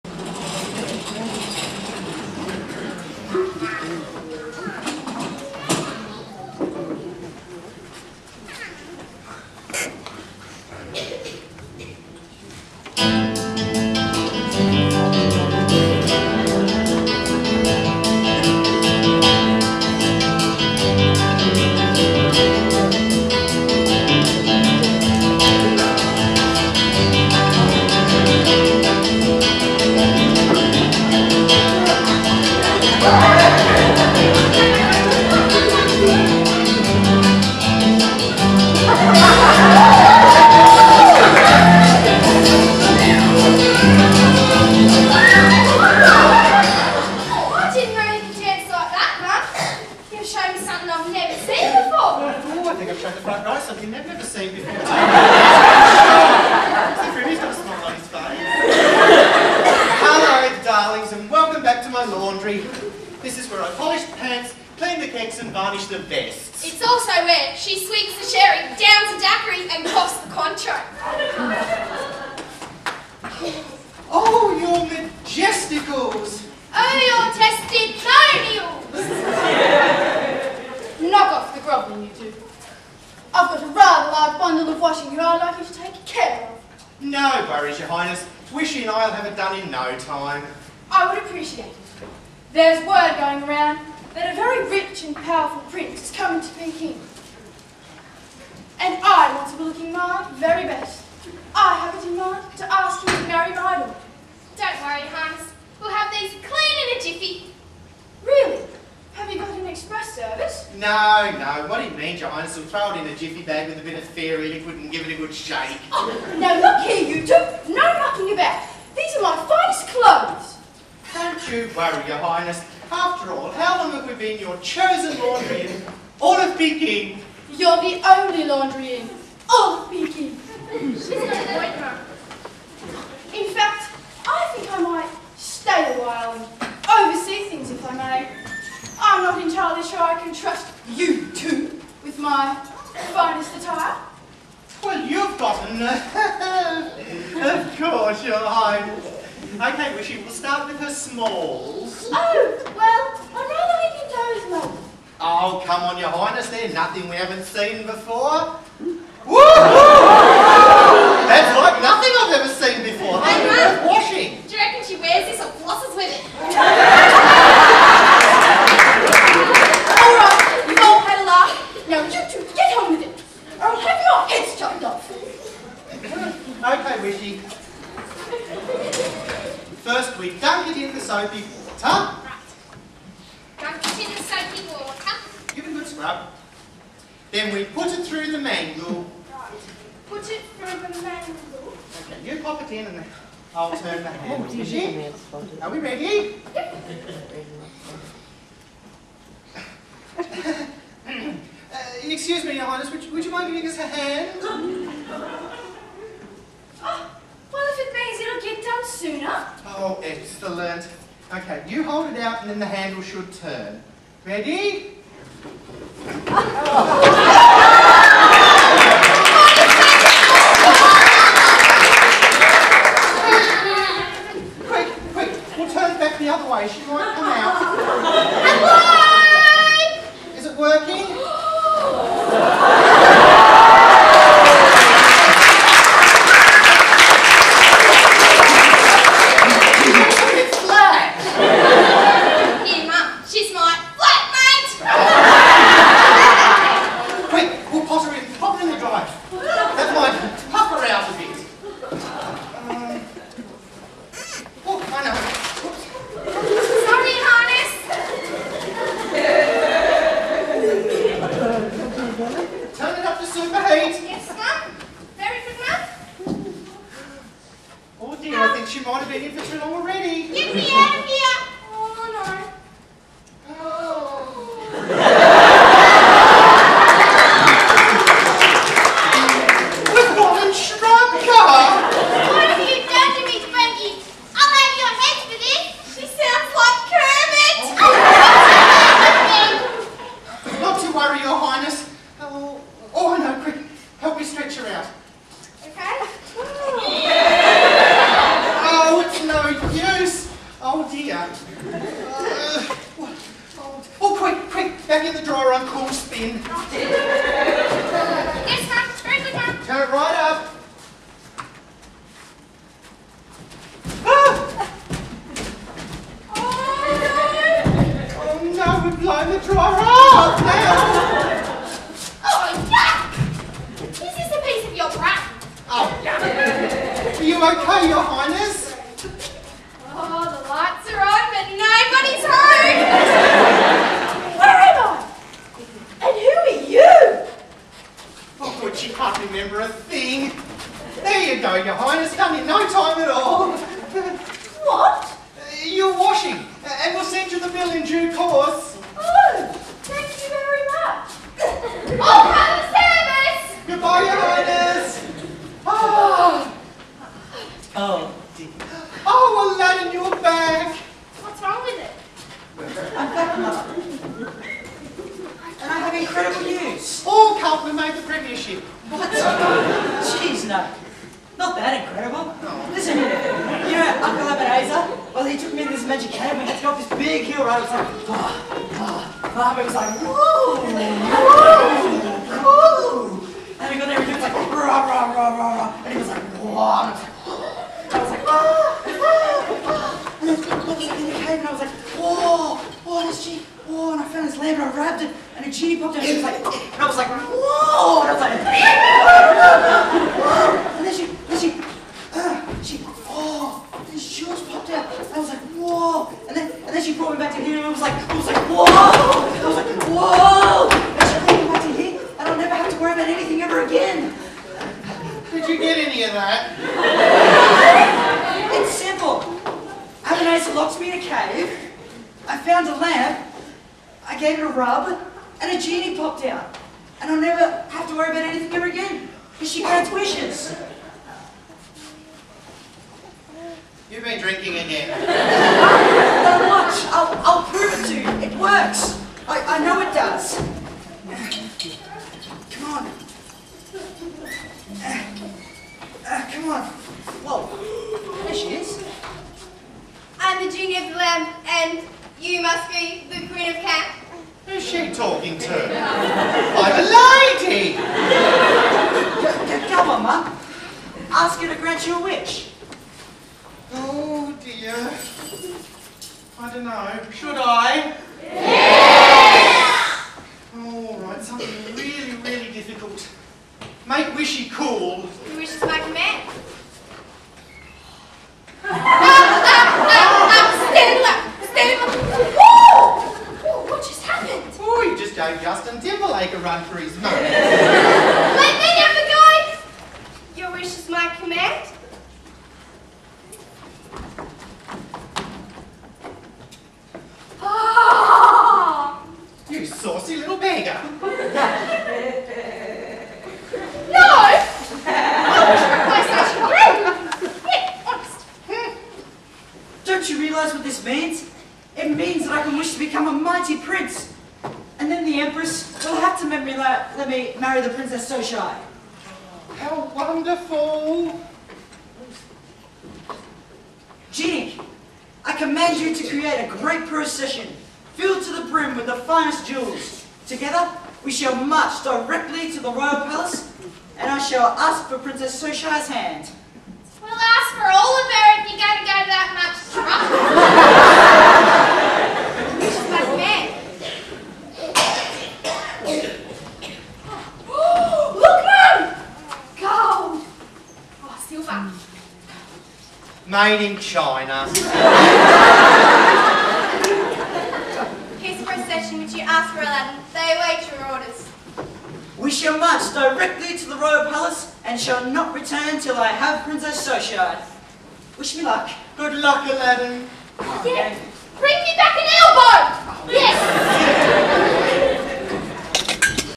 laundry_scene.mpg
ALADDIN script by William Kenning ,Cast EMU BAY PLAYERS Burnie Tasmania. Widow Twankey, Whishee, and Empress of China. Laundry Scene.22nd Nov 2008 Burnie Arts and Function Centre.